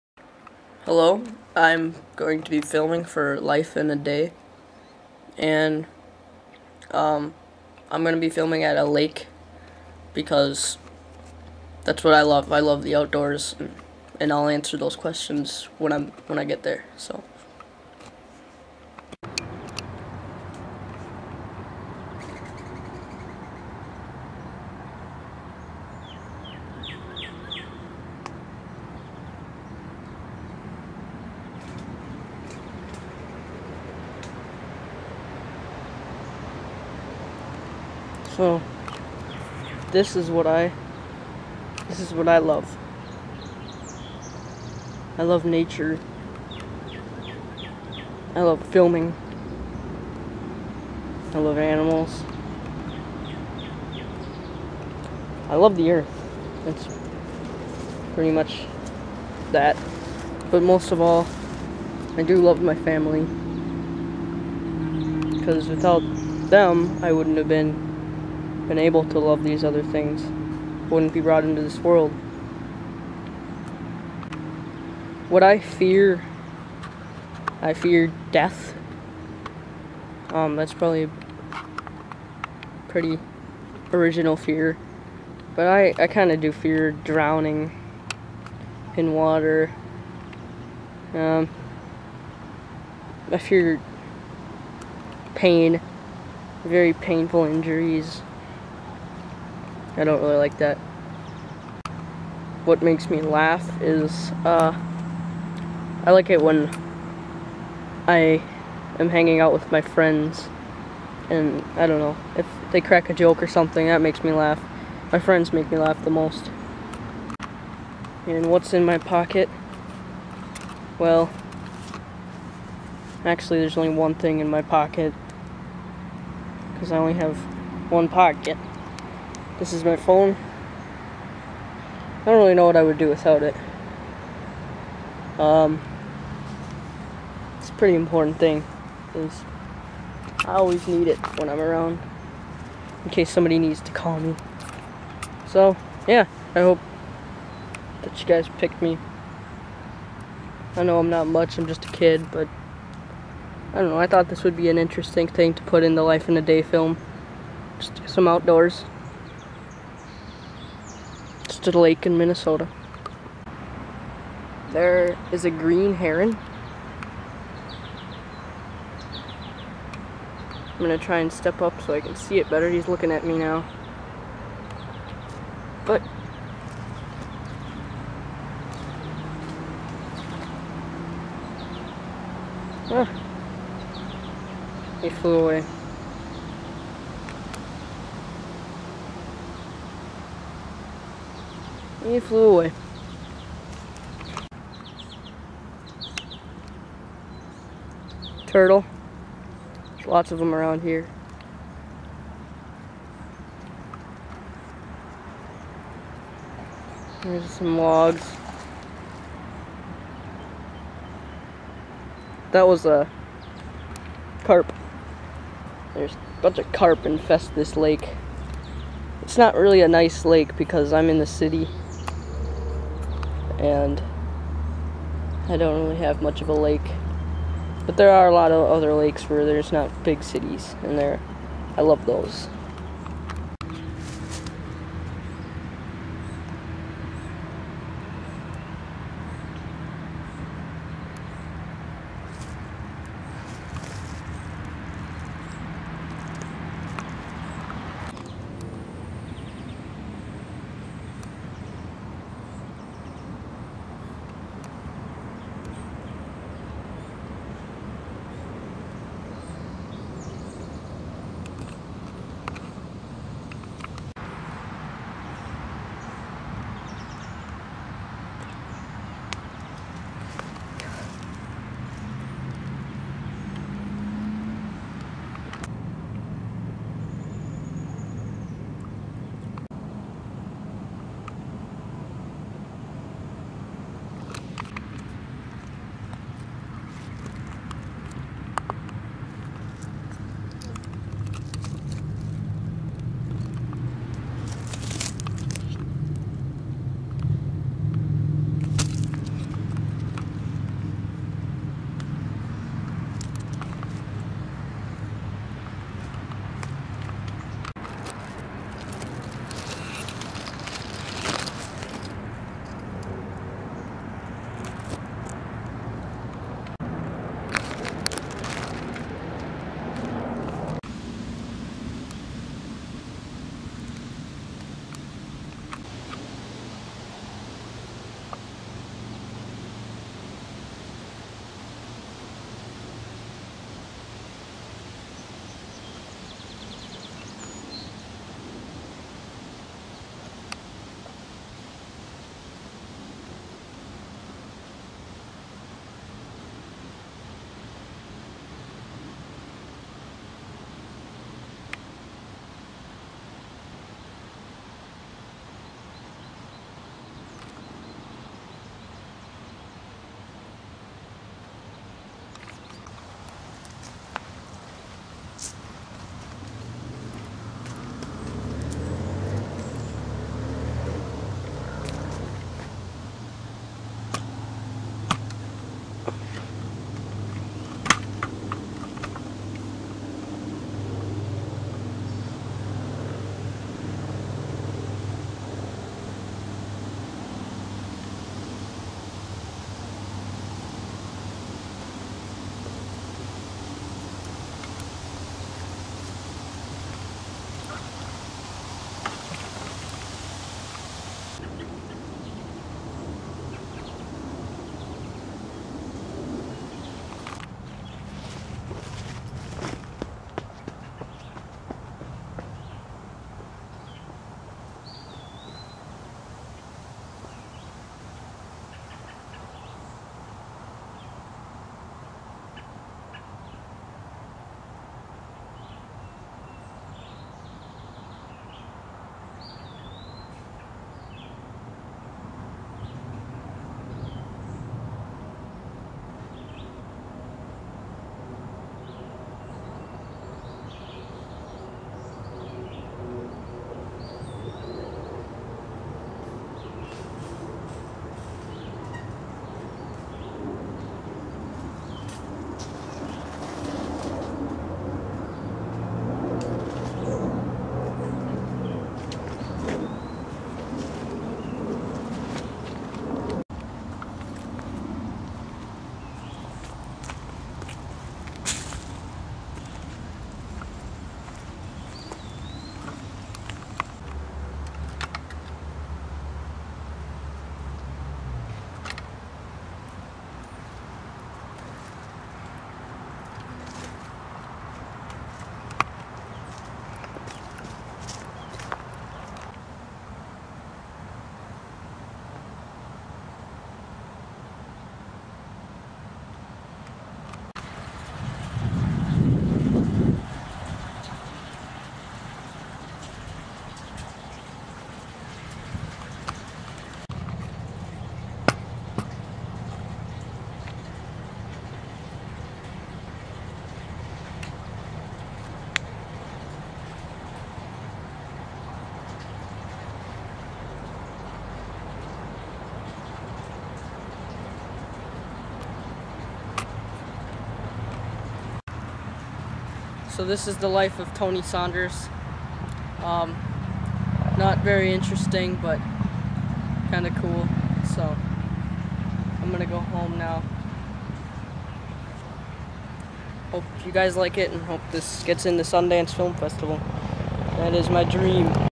My life In A Day
Just a day outdoors at the local lake